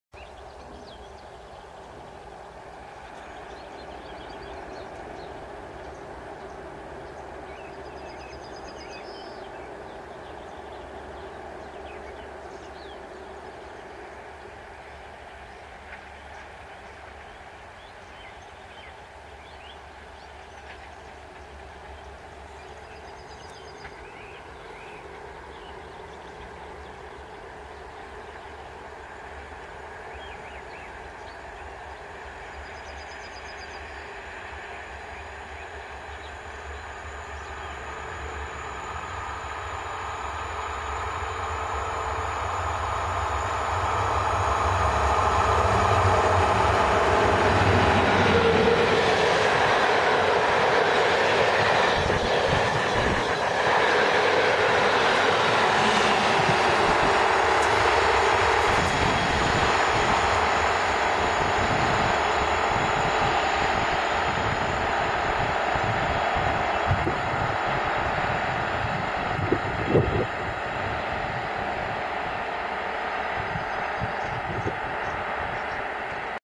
Ostatni kurs TLK 36200 ''Pogórze''
EP07-381 z pociągiem TLK 36200 ''Pogórze'' spółki PKP InterCity w promieniach słońca ostatni raz opuszcza Jarosław, kierując się ku zachodowi...